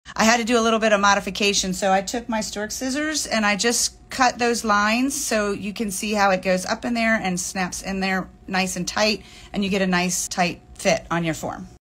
Nail Form Customization | Say goodbye to that annoying product ledge!
Think of forms like puzzle pieces 🧩

Often they have to be customized a little to fit the nail perfectly, but sometimes it’s as simple as this! Measure the width of the nail on the form and cut two diagonal lines outwards to relieve any pressure caused by the sidewalls to get a nice, flush fit between the form and the free edge.